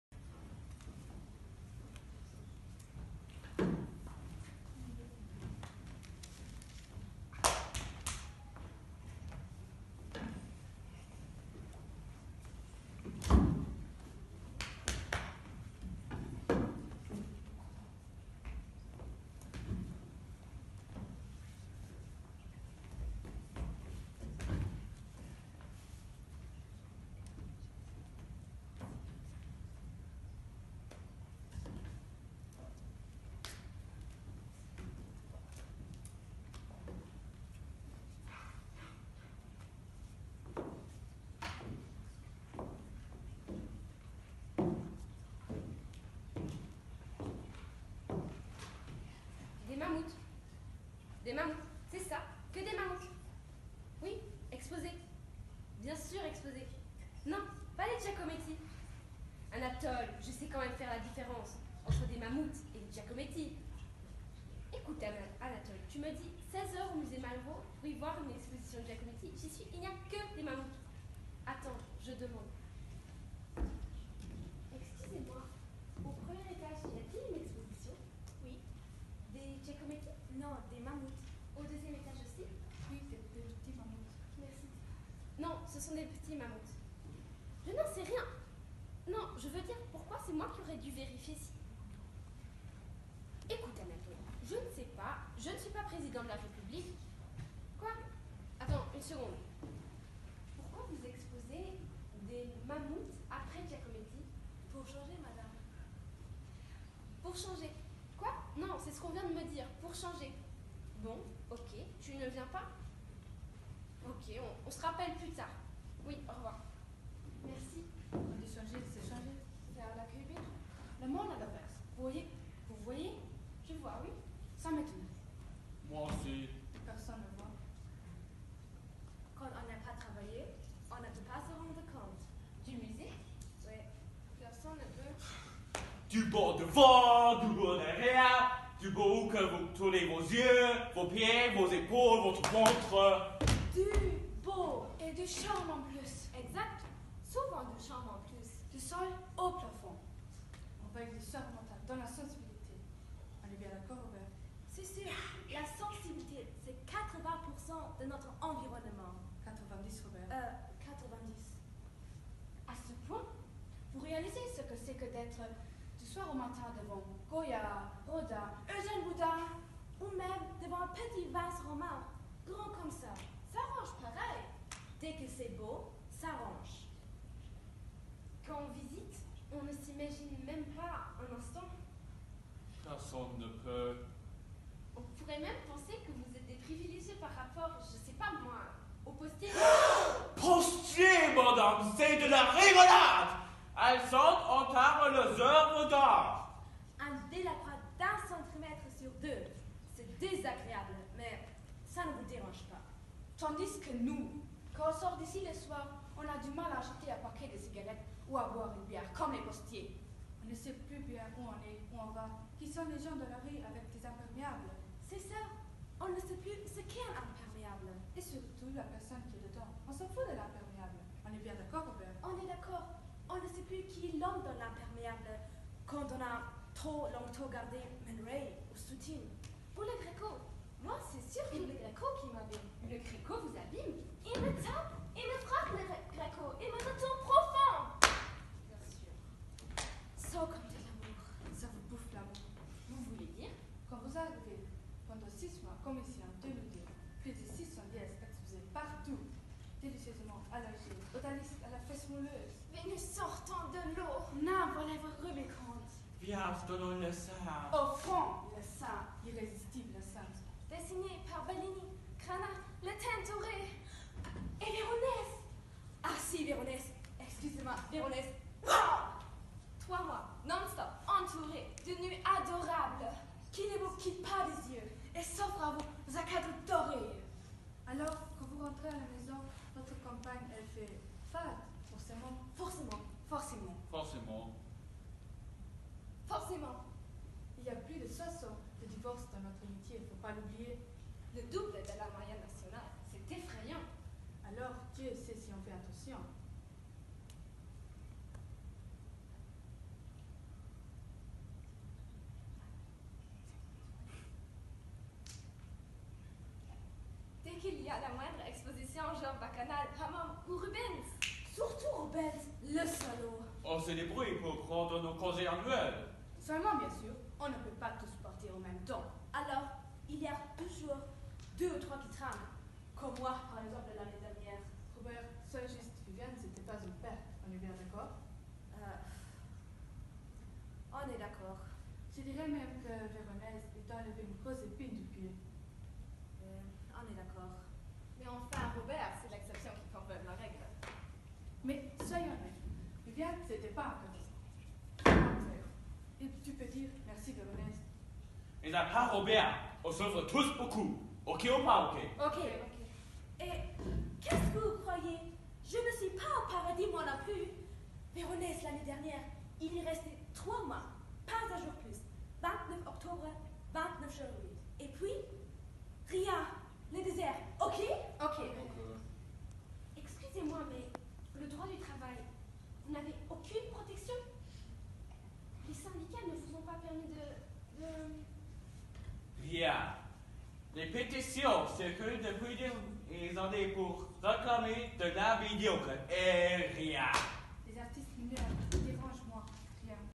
Finale concours de Théâtre Grand Boube
Lors de la finale du concours de théâtre organisé annuellement par la Compagnie du Grand Boube, nos élèves ont présenté sur scène un extrait de la pièce « Musée haut, musée bas »  de Jean-Michel Ribbes.